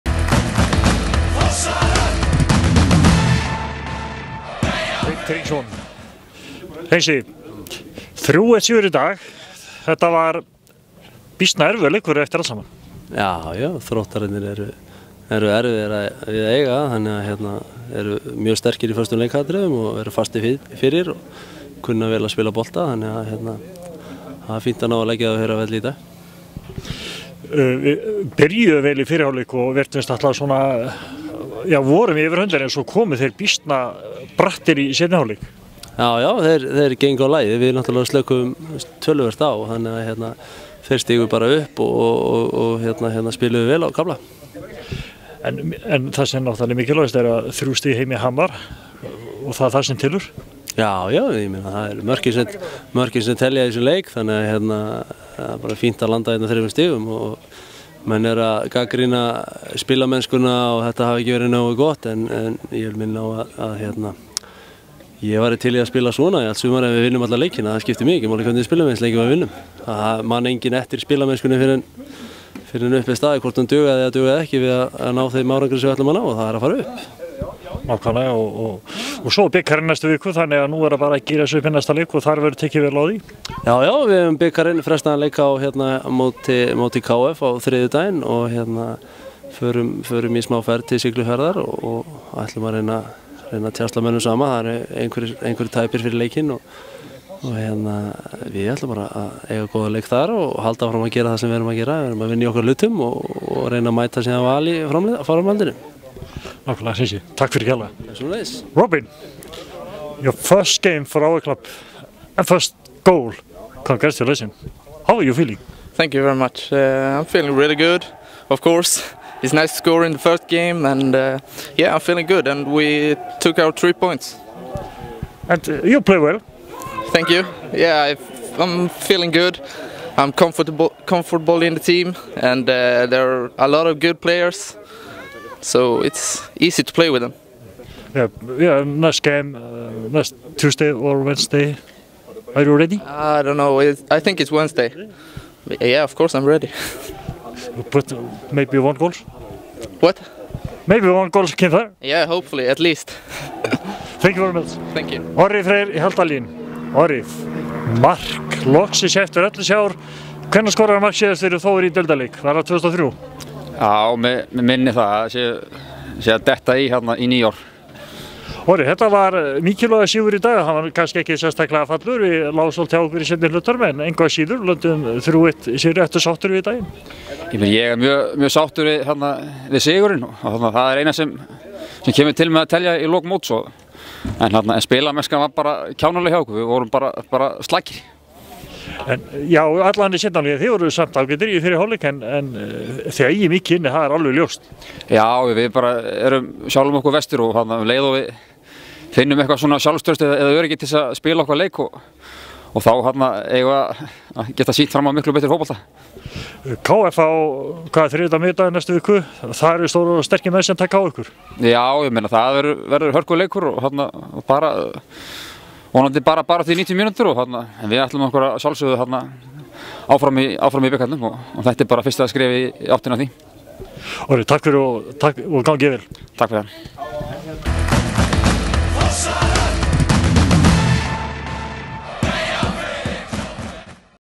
Eftir leik gegn Þrótti maí 2012
Hreinn Hringsson, Robin Strömberg og Orri Freyr Hjaltalín eftir leik Þórs og Þróttar.